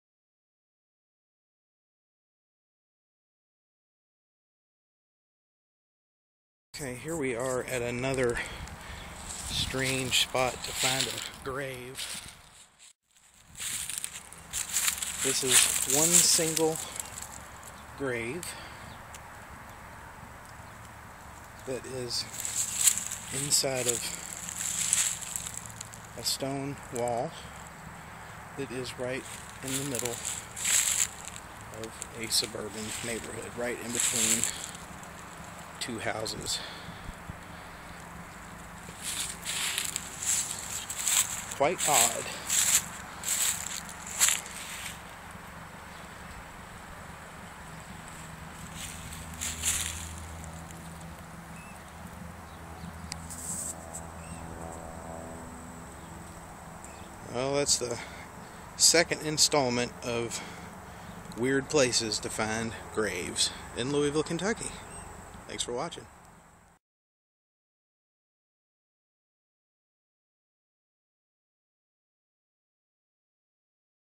FLOYD PARKS GRAVE
A single historic gravesite in an east Louisville suburban area.